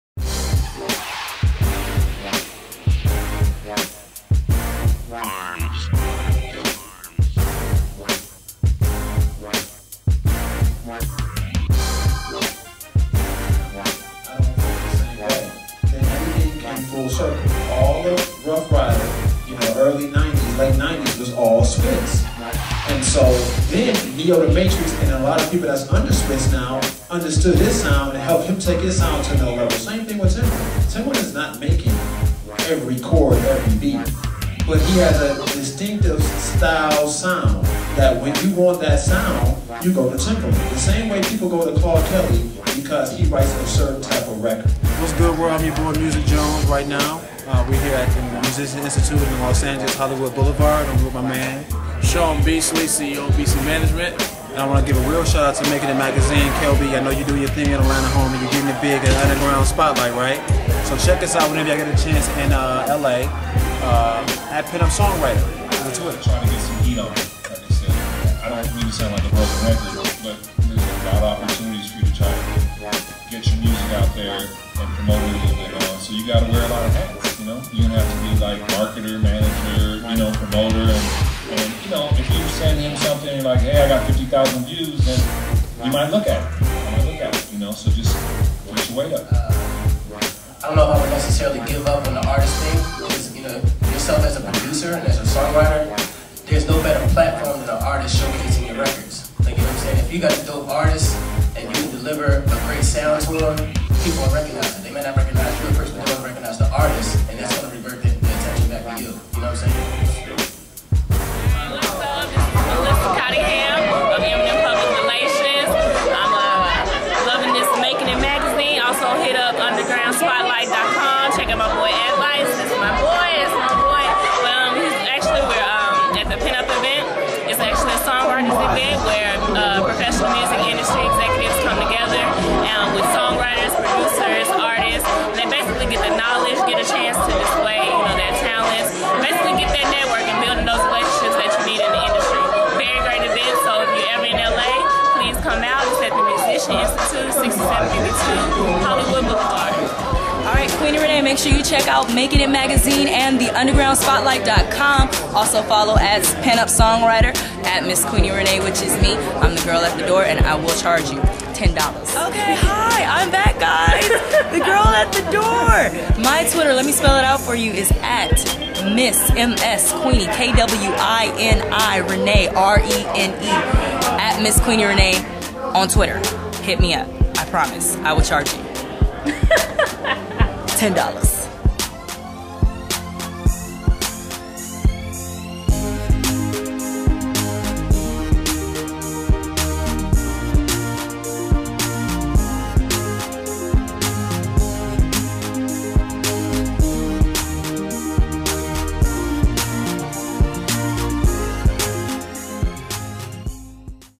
Pen Up: A Networking Event for Song Writers Official Commercial
Check out the Official Promo video for Pen Up: A Networking Event for Song Writers held at Musicians Institute in Los Angeles.  This monthly gathering brings songwriters, producers, emcees, and industry profesionalls together to build better relationships.  for more info on upcoming events.